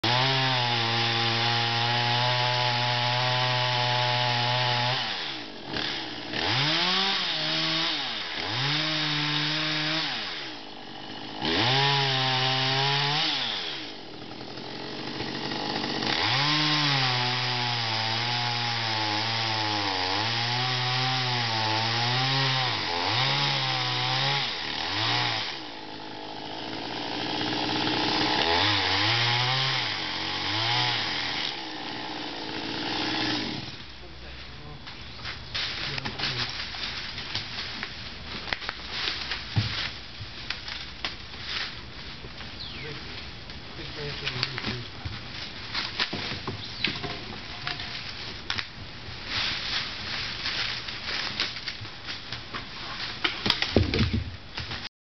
Professionals Pruning Large Trees
Green Heritage Tree Service Ltd. for Halifax, NS pruning our 2 hurricane damaged 55 ft Norway Maples.